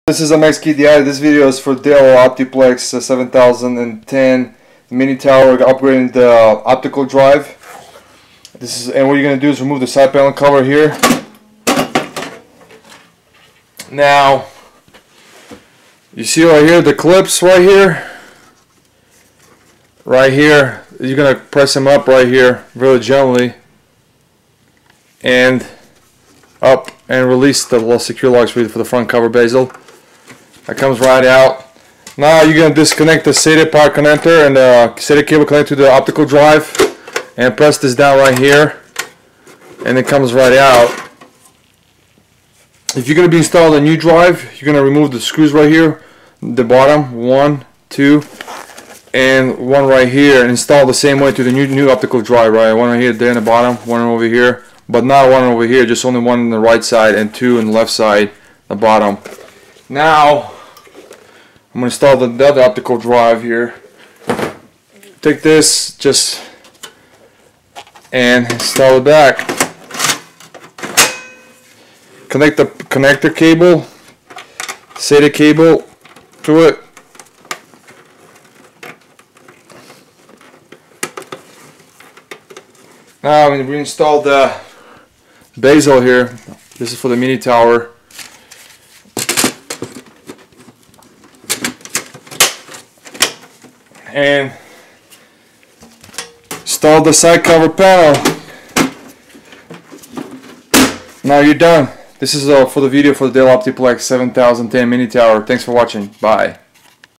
Dell Optiplex 7010 DVD Drive Install Replace Upgrade Change
How to Install Replace Upgrade Change Optical Drive, DVD Drive in a Dell Optiplex 7010.